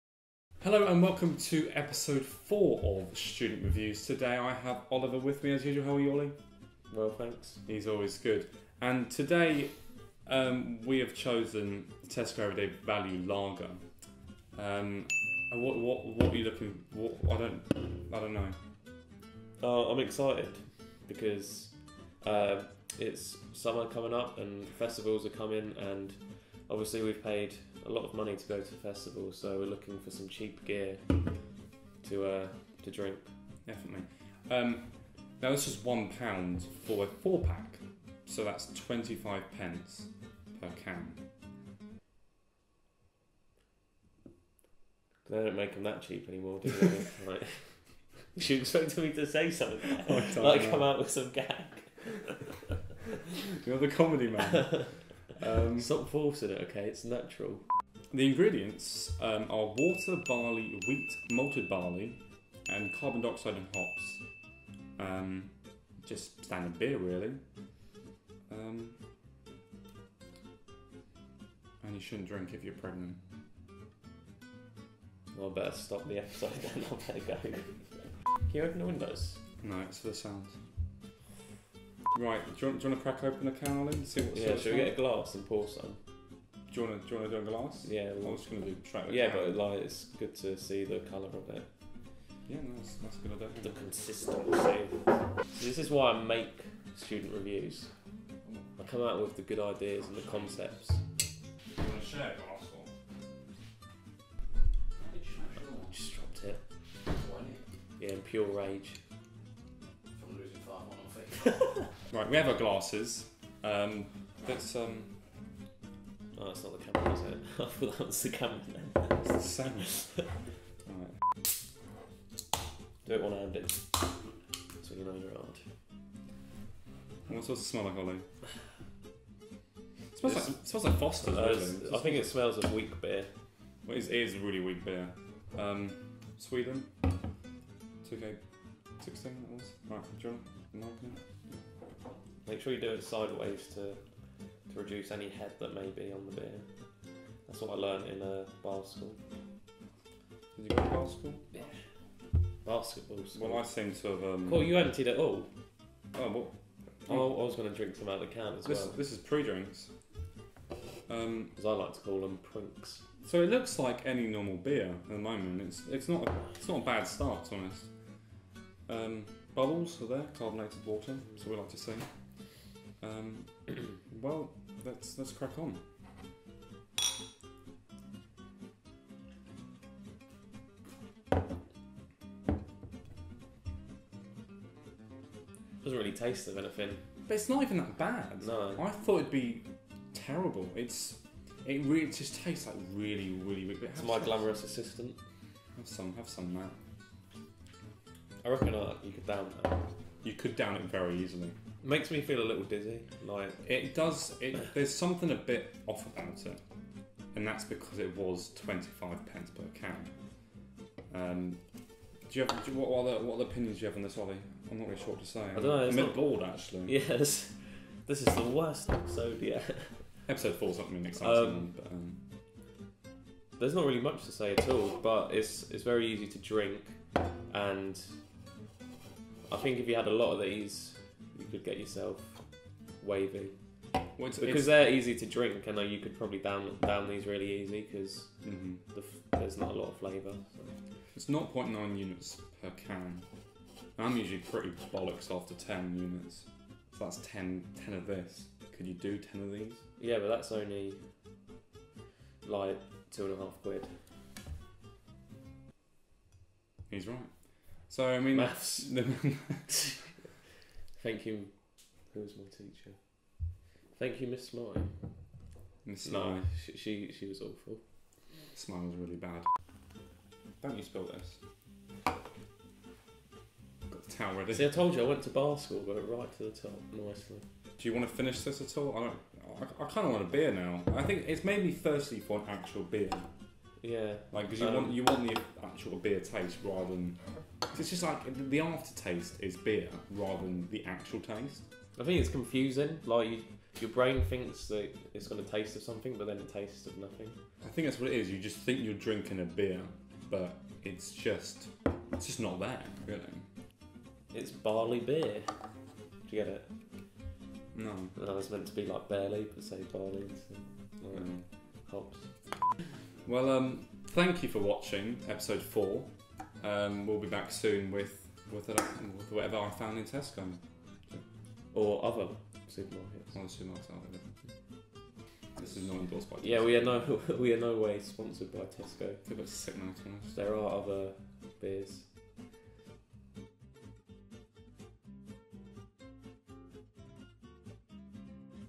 Student Reviews Episode 4 - Tesco Everyday Lager
Today Phil and Ollie try another cheapo. Tesco Everyday Lager. This 4 pack cost £1 and was rather embarrassing to buy.

Starring -
Phil Thompson
Ollie Jordan

Camera - Matthew Oxley
Sound - Phil Thompson
Edited by - Phil Thompson